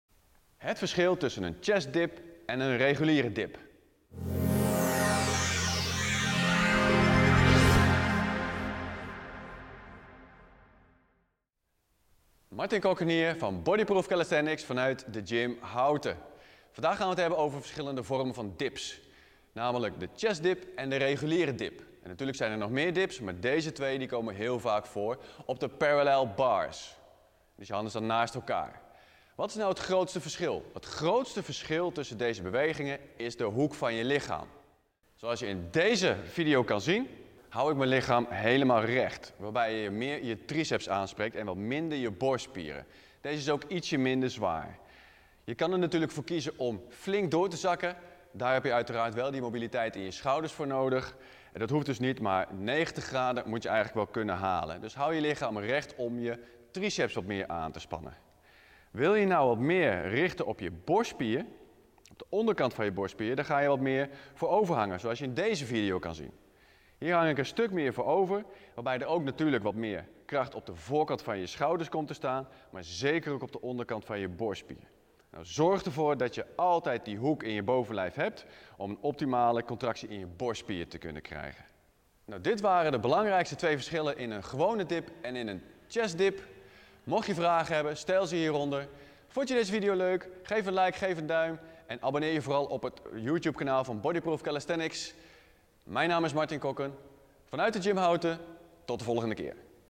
Verschil tussen Chest Dip en Regular Dip
Het grootste verschil tussen de Chest Dip en de Regular Dip is de hoek van je bovenlichaam.
Is je bovenlichaam verticaal, dan focus je meer op de Triceps en heeft je bovenlichaam een hoek van ongeveer 30 graden dan focus je meer op de onderkant van de borstspieren.

Probeer het eens en voel het verschil!

Succes met trainen en laat het me weten als je vragen hebt!